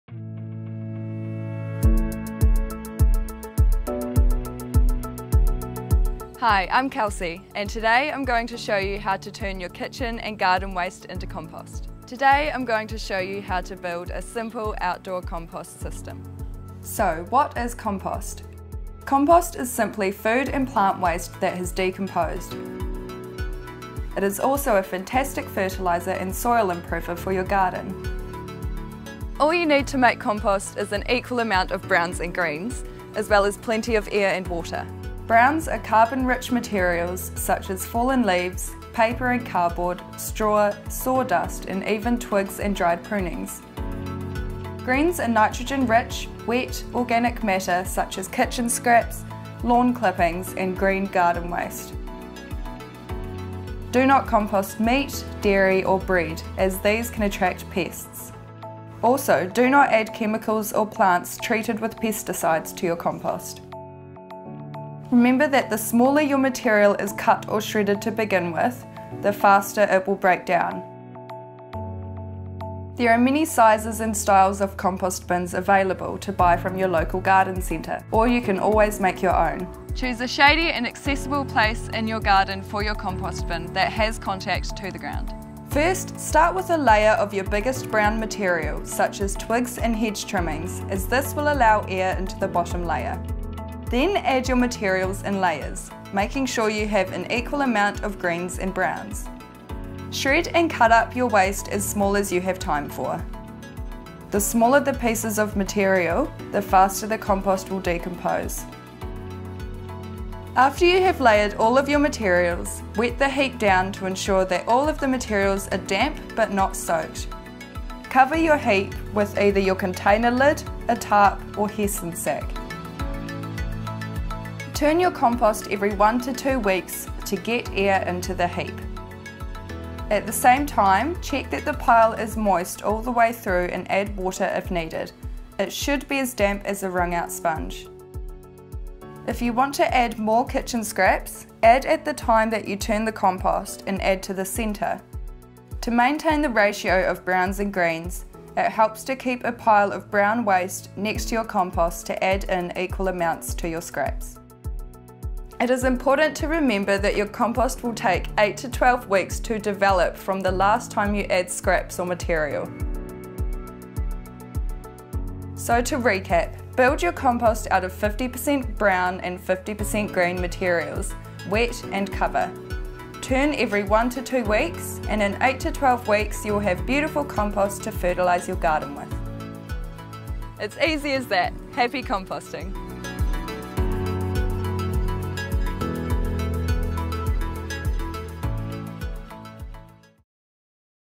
Beginner's Guide to Composting
This Nelson City Council video looks at how to set up a household compost bin.